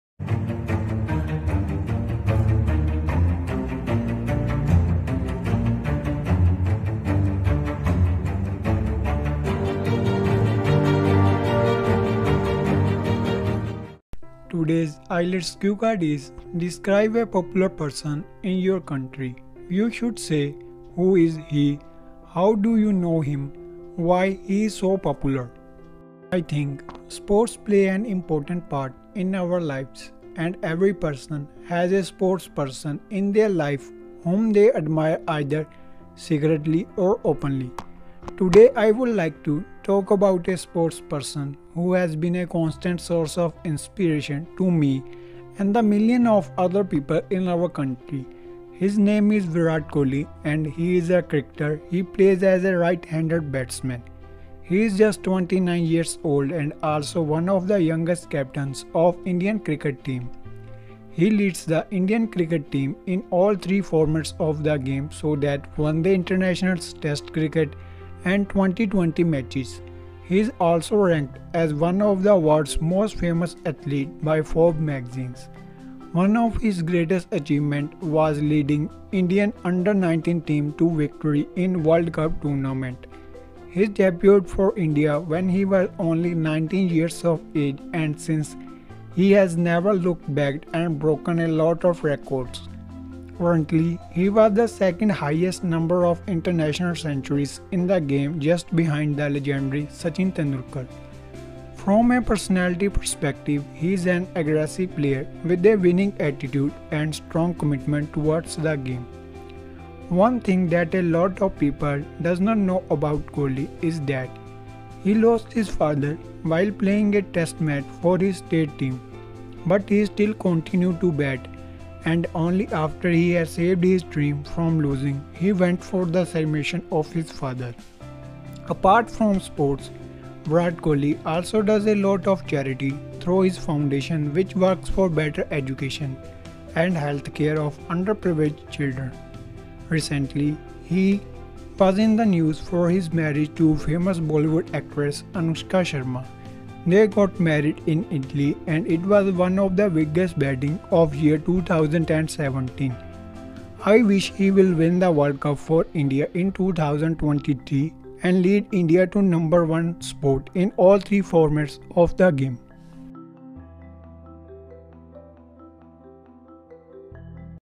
Describe a popular person in your Country | IELTS Speaking Latest Cue Card Topic
In this video we will come to know about a latest and most important cue card, that is "Describe a popular person in your country."